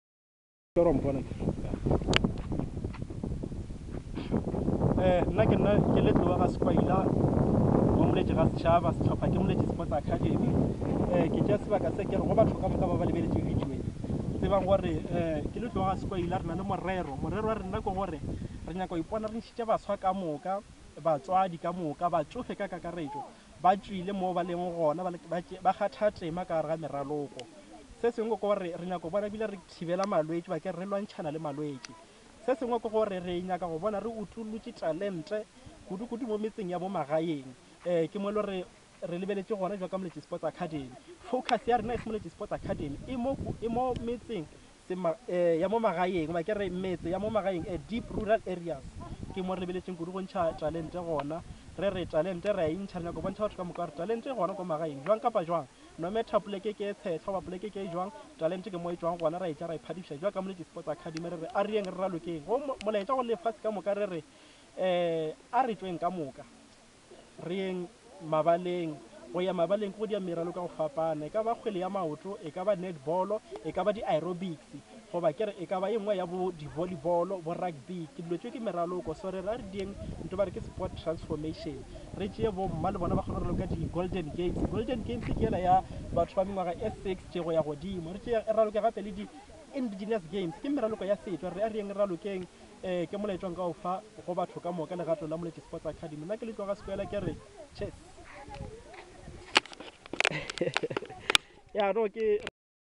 Moletjie got talent, short profile about Moletjie Sports Academy
Moletjie Spots Academy`s chairperson Mr. Tlou Sekwaila talking about our service.